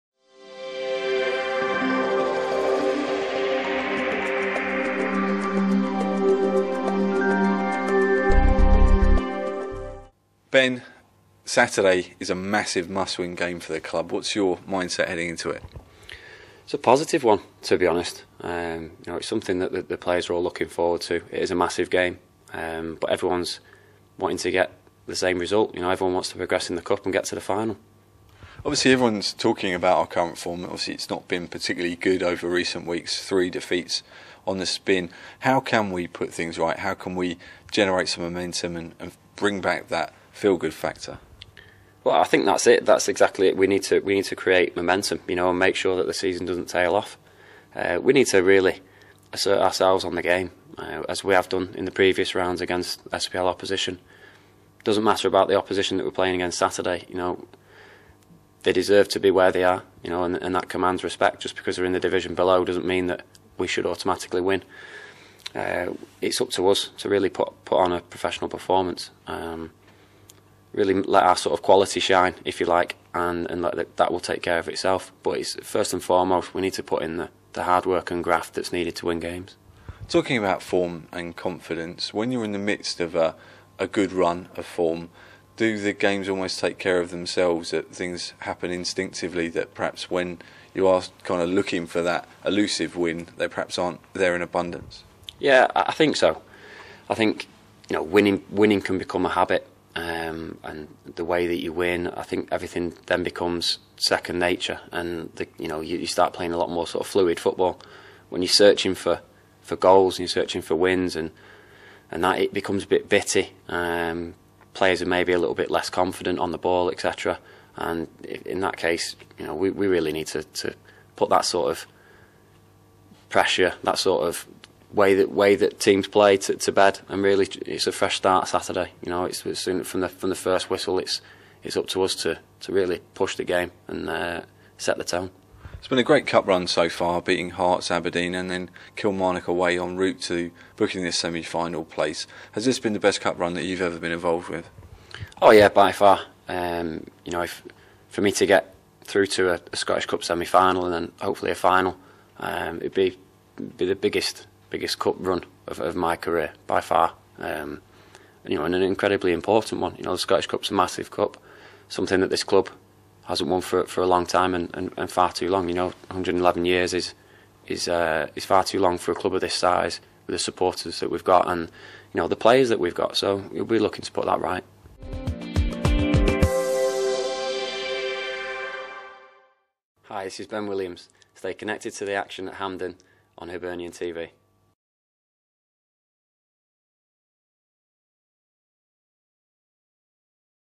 Ben Williams Previews Hibernian v Falkirk
Ben Williams speaks to Hibernian TV via YouTube ahead of the club's William Hill Scottish Cup semi final against Falkirk at Hampden (Saturday 13 April, kick off 12.30pm).